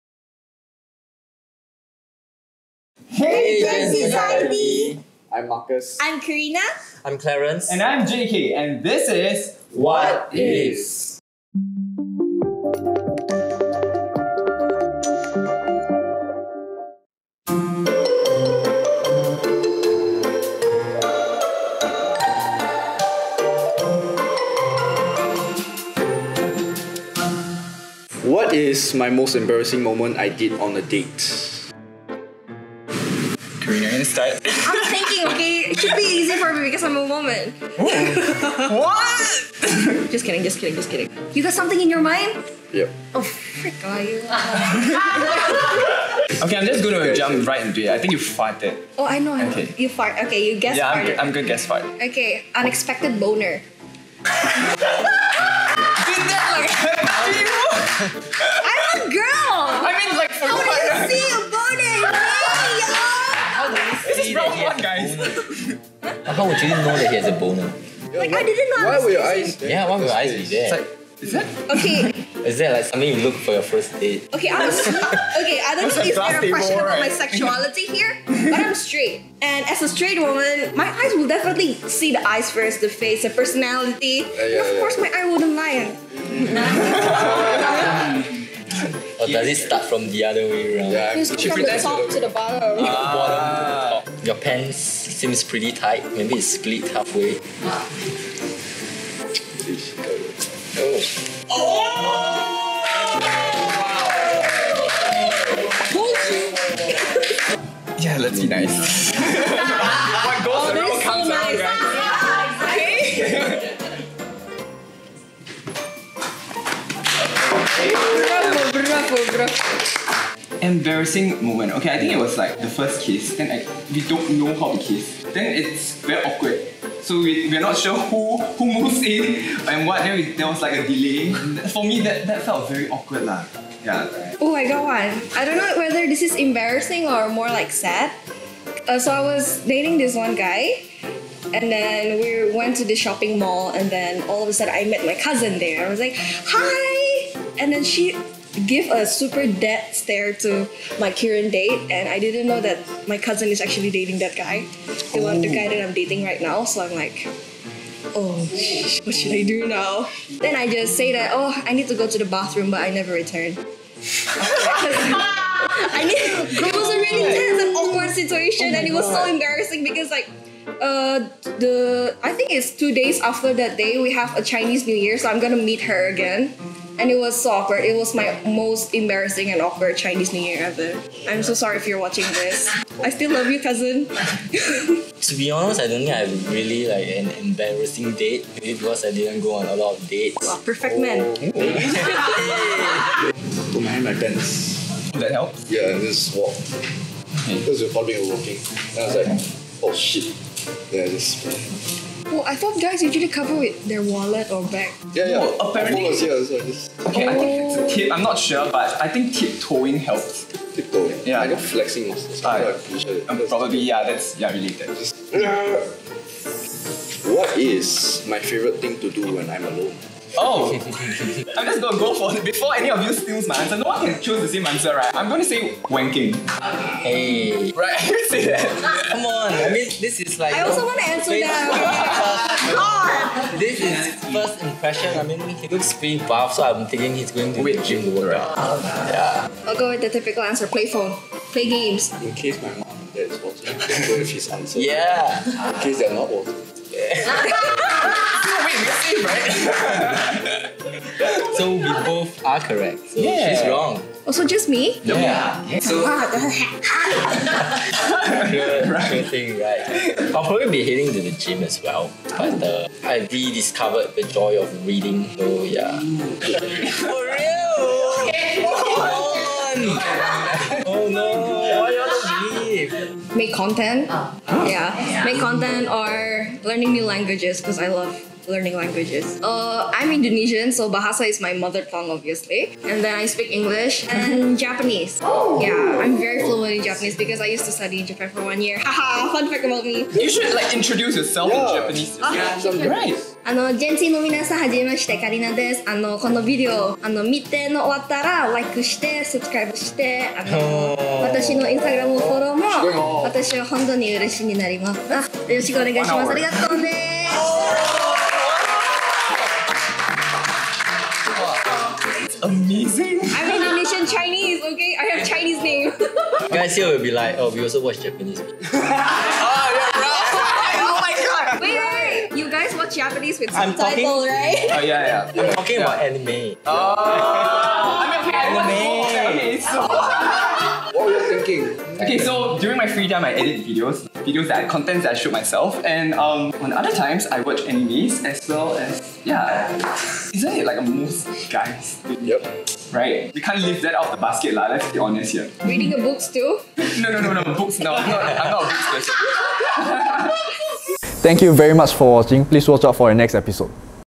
WHAT IS your favorite thing to do when you’re alone? | Gen-Z Magazine
WHAT IS your most embarrassing moment on a date?  and
WHAT IS your favorite thing to do when you’re alone?

Let’s see whose assumption is right! Here’s another fun episode of “WHAT IS.”

Spirit by: Cask Collective
Coffee by: Hook Coffee

Wanna do this fun challenge?